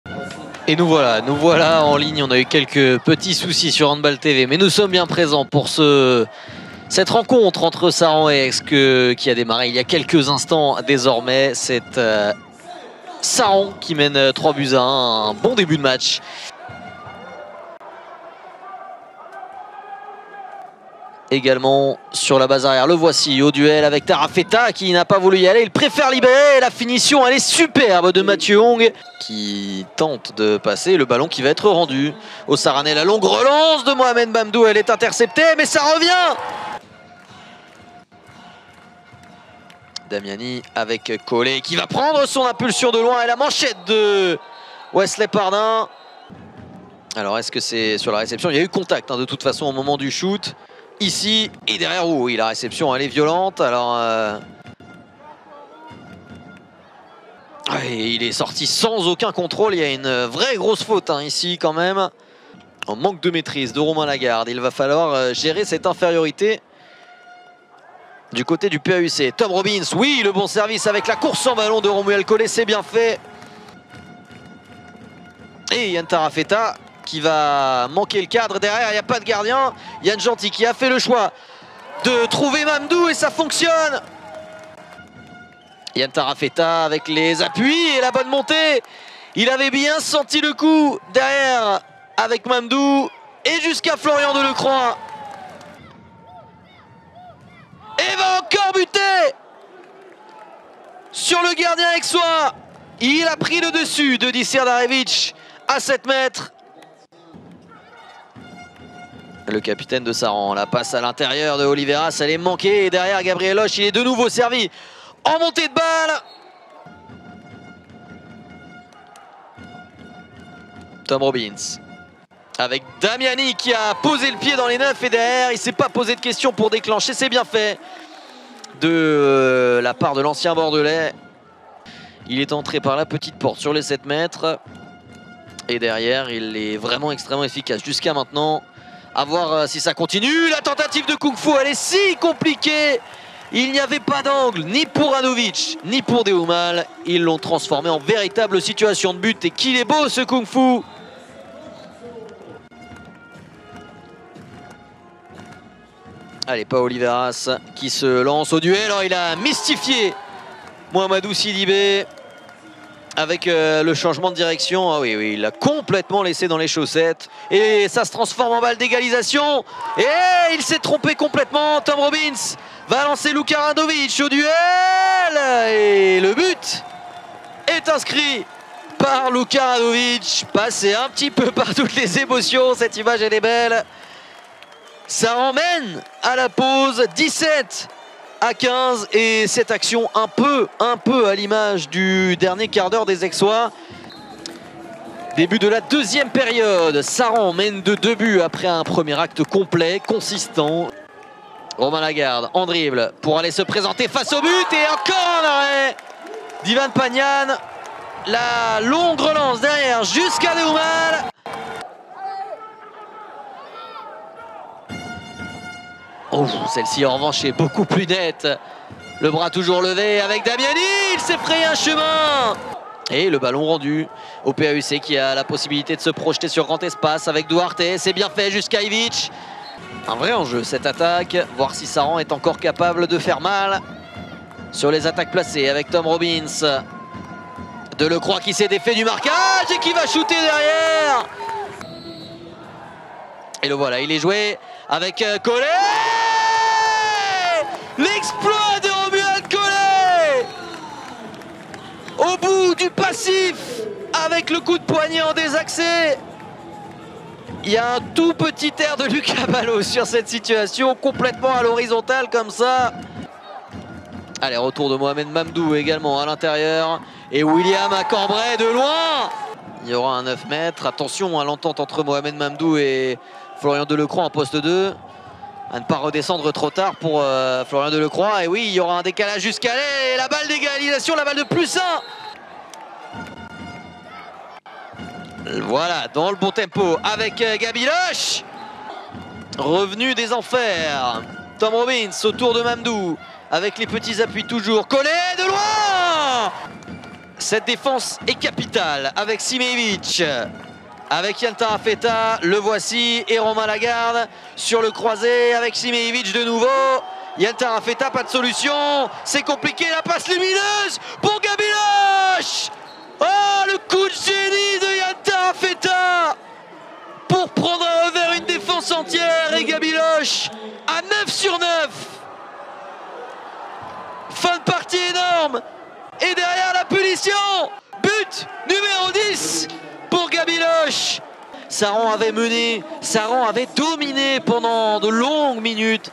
LMSL J19 : SEPTORS-AIX, Le résumé
Retour sur la défaite face à Aix (32-34)